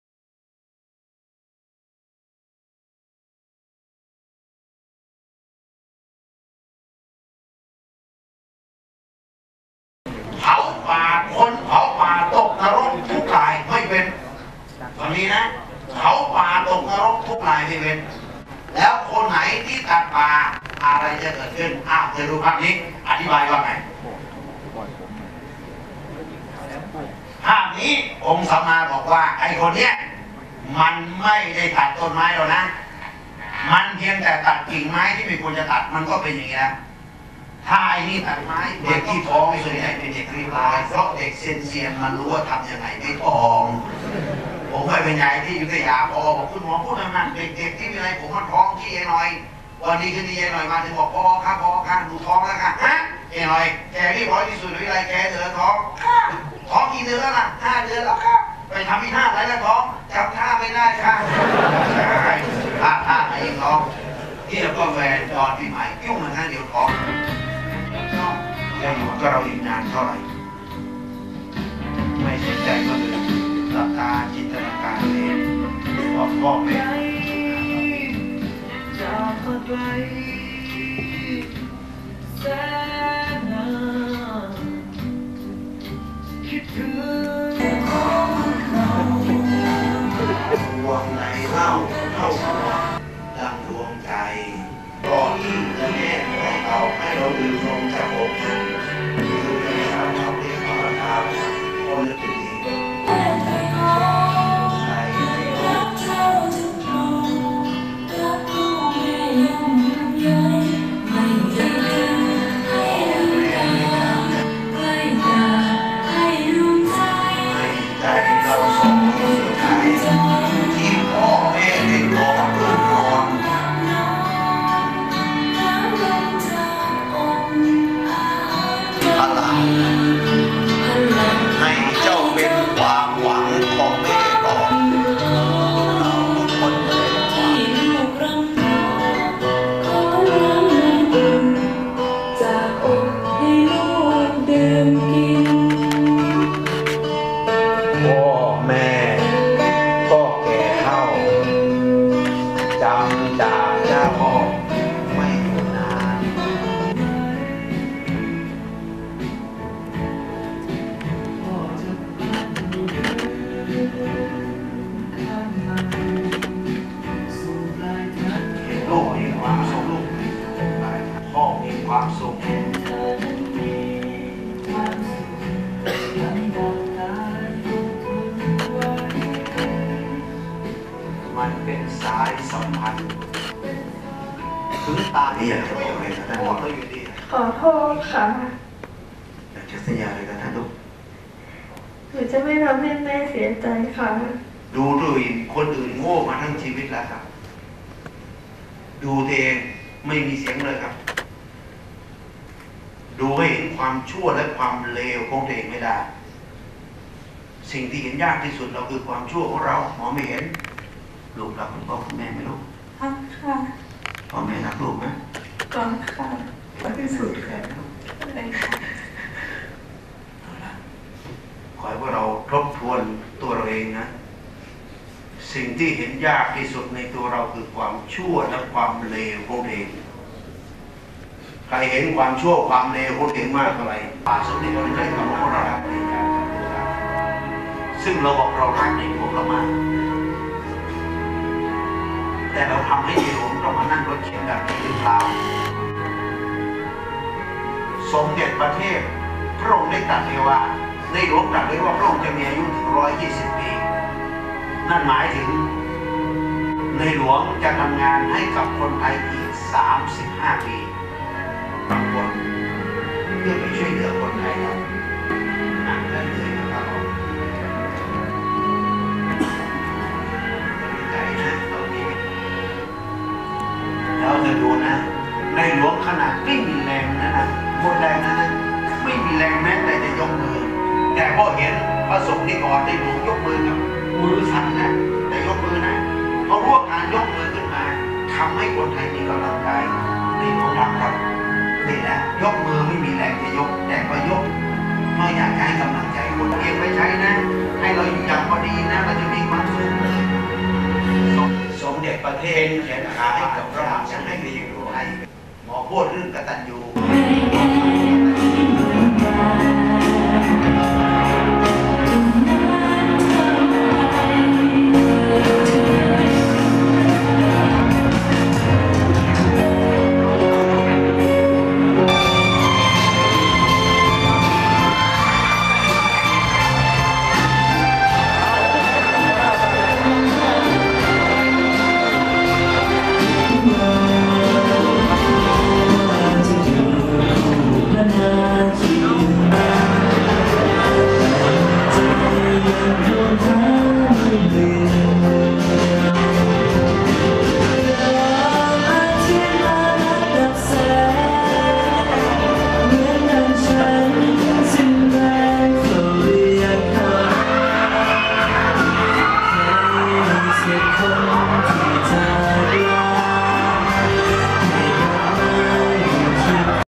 คลิป วัน ปัจฉิมนิเทศน์ ปวส.2 โปลิ
คลิป วัน ปัจฉิมนิเทศน์ นศ. ปวส.2 วิทยาลัยเทคโนโลยี โปลิเทคนิคลานนา เชียงใหม่
วันที่ 14 ก.พ. 2556